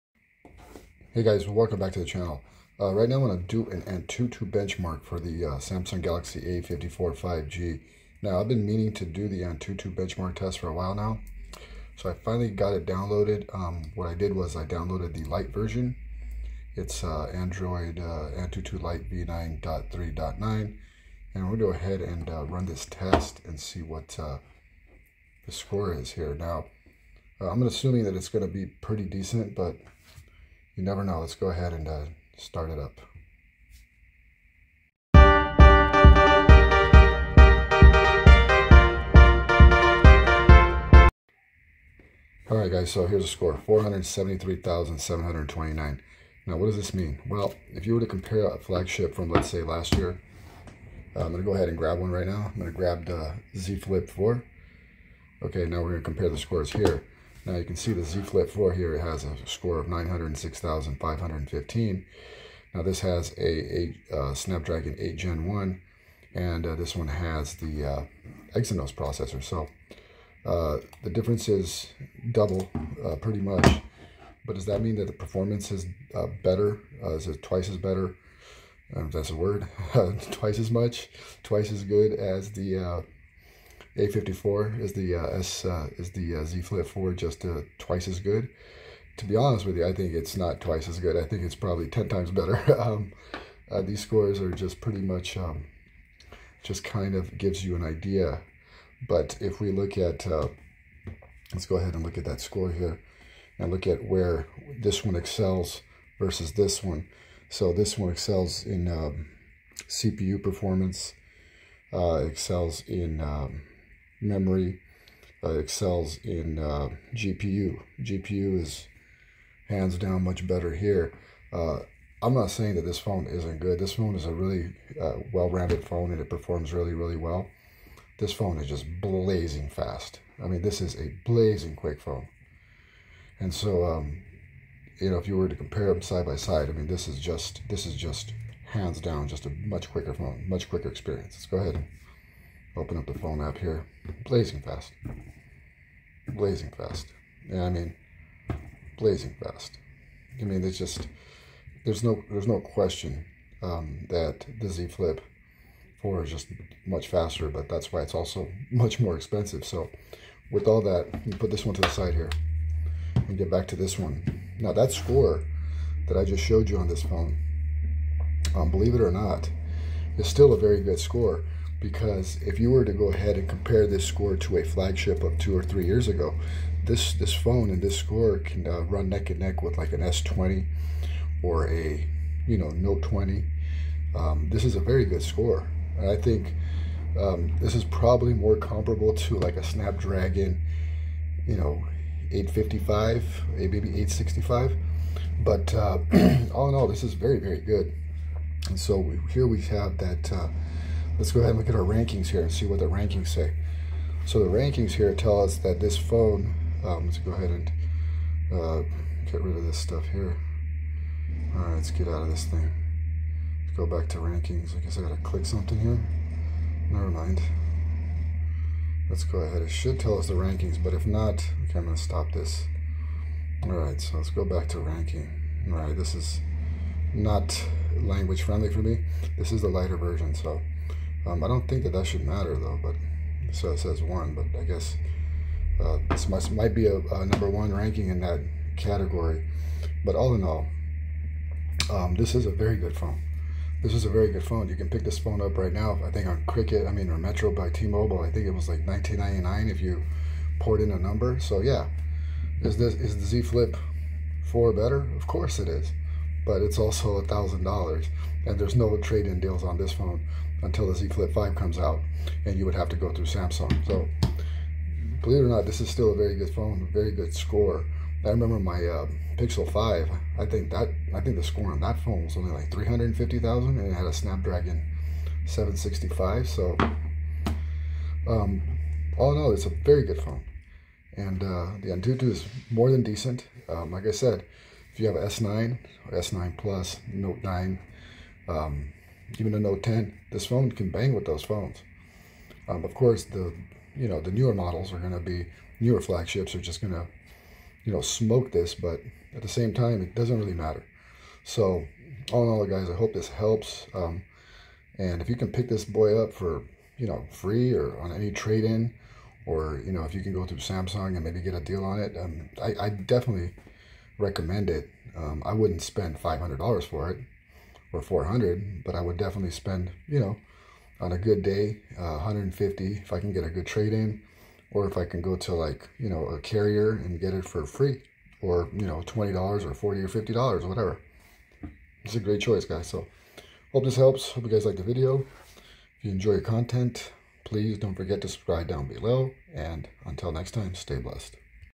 Samsung A54 5G Antutu Benchmark . How does it compare to the Z flip 4? 🤔
Antutu Benchmark Samsung A54 5G